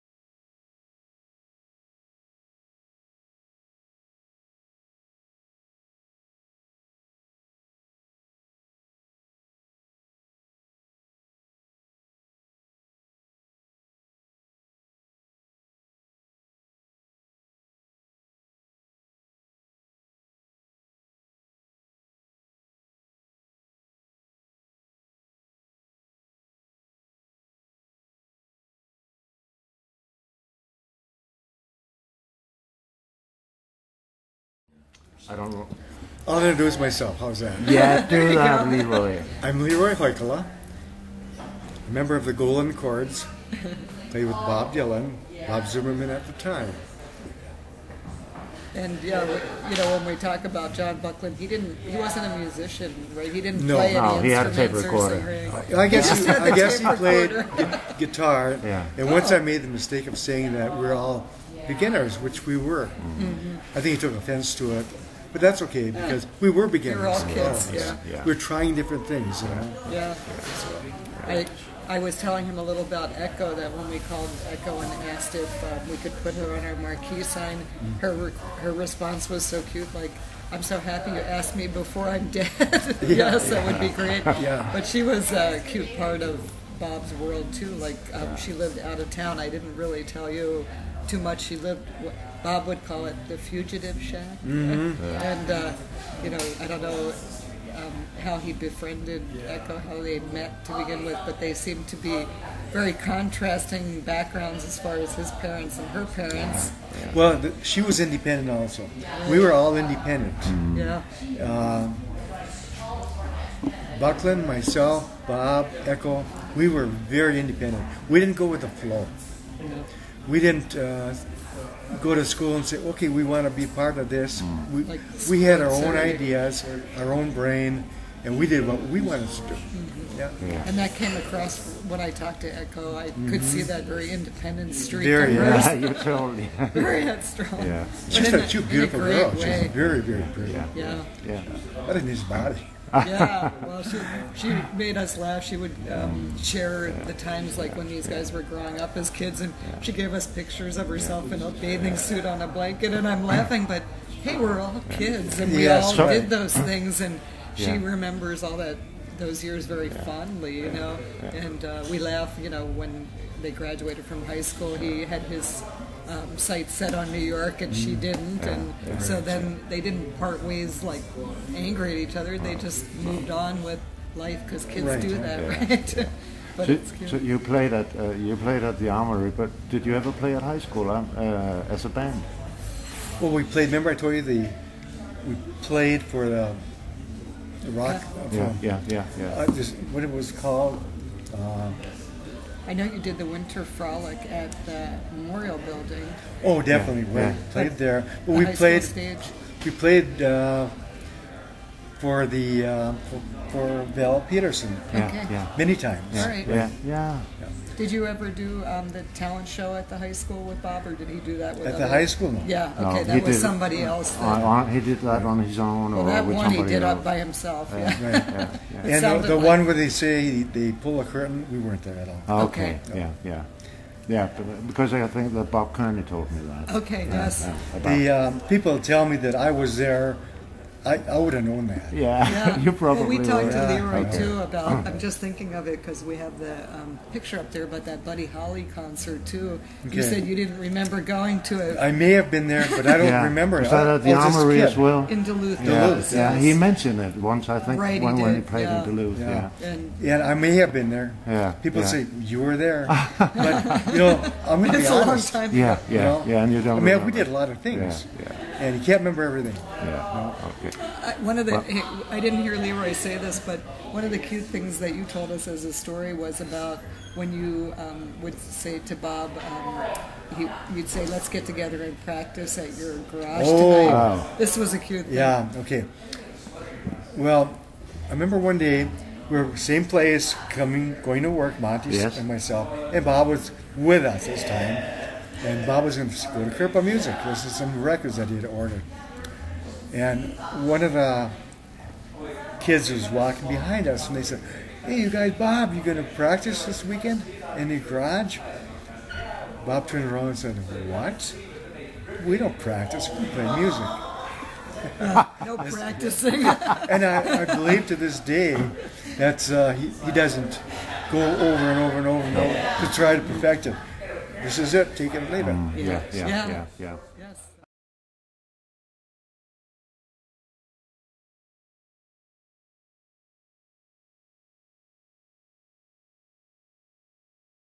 A brief interview with LeRoy Hoikkala & Linda Strobach about Bob Dylan and their early friendship.
I spent a few days in Hibbing Minnesota last october  with my eldest son Jakob. We visited Zimmy´s, the Bob Dylan cafe at main street and were met by Linda the owner, who immidiately called LeRoy Hoikkala. Listen to this conversation about days long gone and about Bob Dylan. I´m so fond of having met these two nice people.